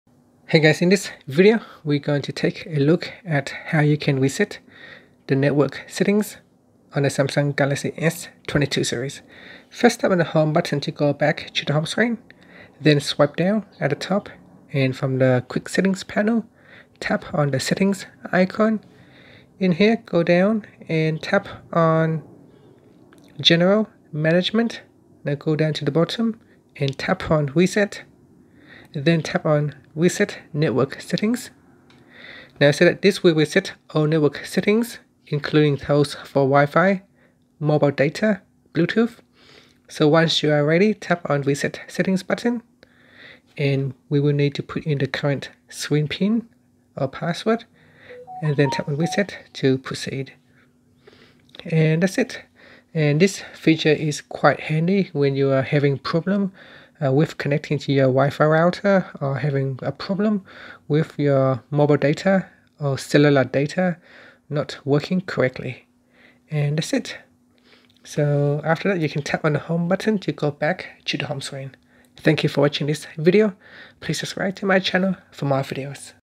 Galaxy S22/S22+/Ultra: How to Reset Network Settings
Learn how you can reset the network settings on the Galaxy S22/S22 Plus/Ultra. Reset the Network settings can help you fix problem with wifi connection and cellular mobile issues.